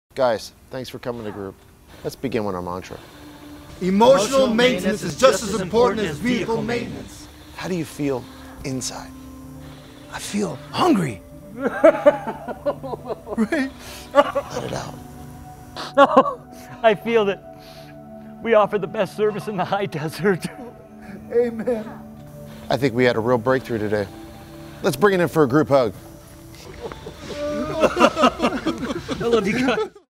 Valley Hi Toyota Service Commercial
Toyota Todd visits the Valley Hi Toyota Service Center and has his talk with the technicians. They begin with their Mantra:
"Emotional maintenance is just as important as vehicle maintenance."

Our Service Department prides itself on providing the best service in the High Desert. Sometimes, your visit is longer than usual, but our goal is to get your vehicle back on the road in its best condition possible.
Our new Service Advisor, Mike Purpura is adamant about serving the Victor Valley! Our main goal is customer satisfaction and to build trust with our friends and neighbors.
This is a light-hearted commercial showcasing a couple of of very own service techs (Chuck McBurney and Andrew Cortez)!